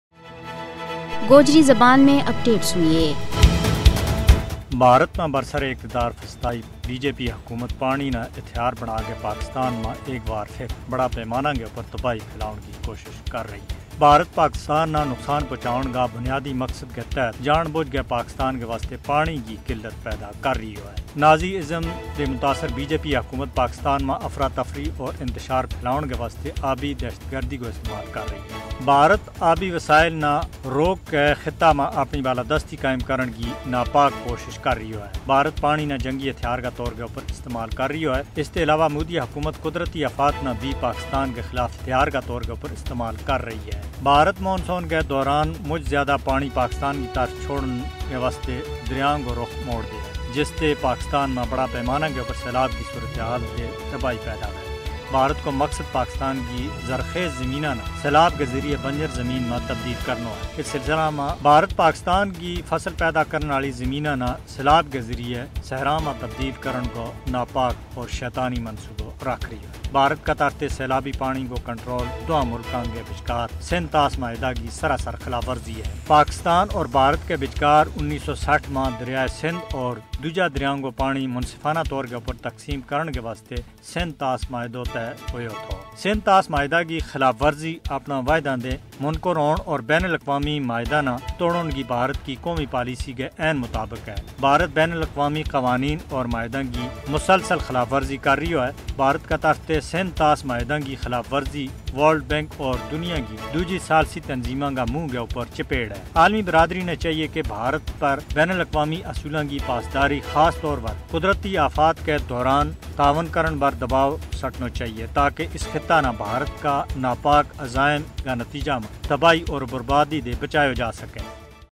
India is using water as a weapon of war to weaken Pakistan || Gojri || Radio SHJ&K
India is using water as a weapon of war to weaken Pakistan . Latest From Radio Sada-e-Hurriyet Jammu and Kashmir.